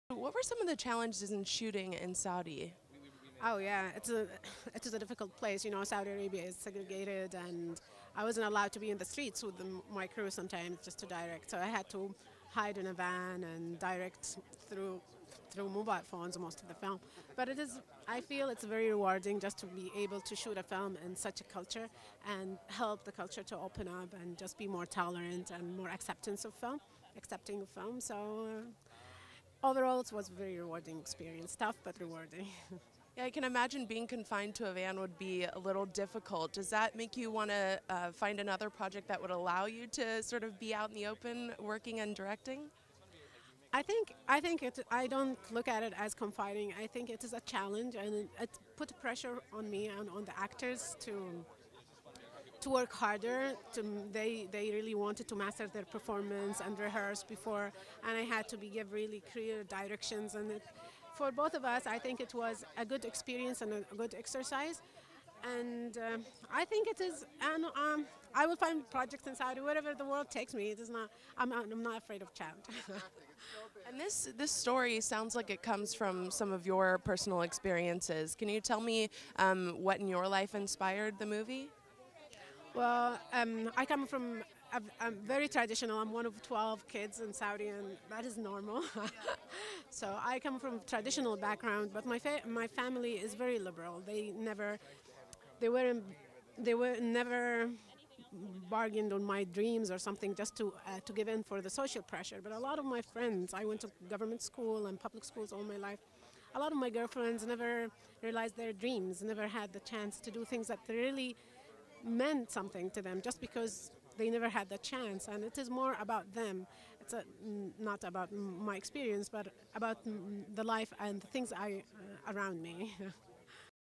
Haifaa Al-Mansour -- Director of 'Wadjda' -- A Beyond Cinema Original Interview
Beyond Cinema spoke to Haifaa Al-Mansour about her powerful portrait of a young Saudi woman -- 'Wadjda'. As a female filmmaker in Saudi Arabia, Al-Mansour was often restricted from interacting with her cast and crew in public durin the production. She tells us about the challenges of shooting a film in the region. 'Wadjda' stars Reem Abdullah, Waad Mohammed and Abdullrahman Al Gohani. Haifaa Al-Mansour was honored at Variety's Indie Impact Brunch as a part of the publication's "10 Directors to Watch" list.